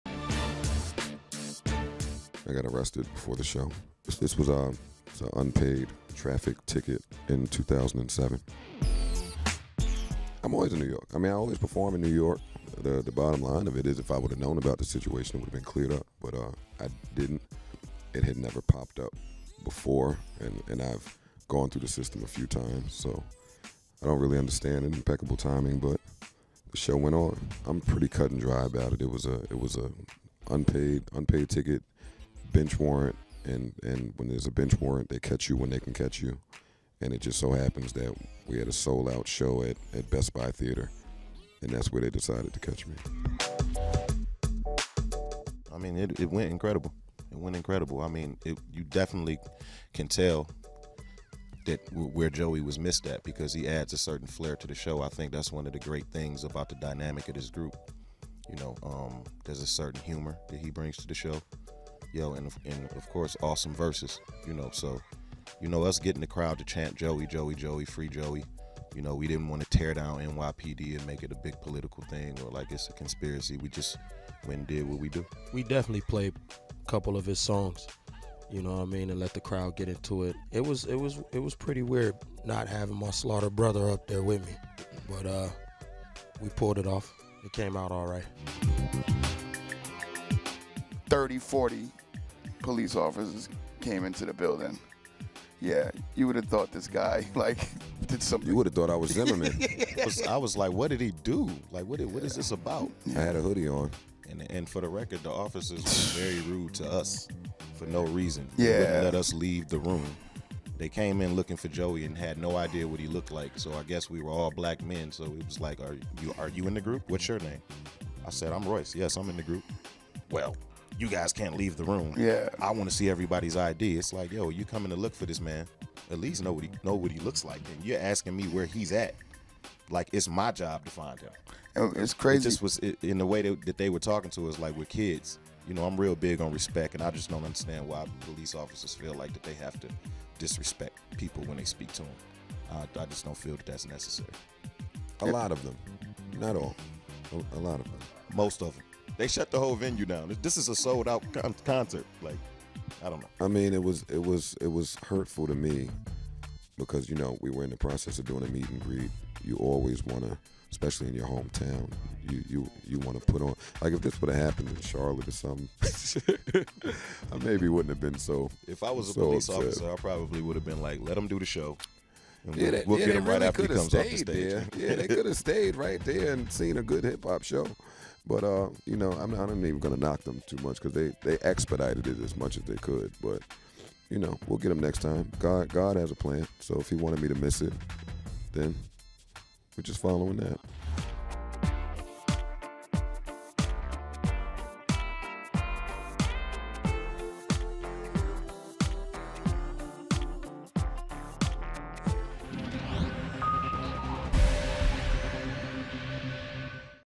Slaughterhouse on Joe Budden Arrest
Rap supergroup Slaughterhouse discuss Joe Budden's arrest just before their March 29 concert at New York's Best Buy Theater.